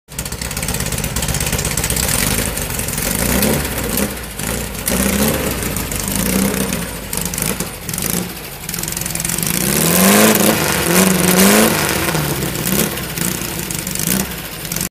VW dune buggy and trailer for sale at auction | bidding closes May 22, 2019
VW dune buggy and trailer
Four cylinder gas engine
IRS transmission
Lights
Aluminum wheels
6.80-15 smooth front tires
13.50-15 paddle track rear tires
(4) Cooper Cobra P275/60R15 tires
(3) helmets
2011 PJ 7712 tilt deck trailer
15' overall length
80"W
Wood deck
2" ball hitch
Spring suspension
ST205/75R15 tires
4P5U71215B2160039
It's located in Guymon, OK, and we're selling it to the highest bidder regardless of price.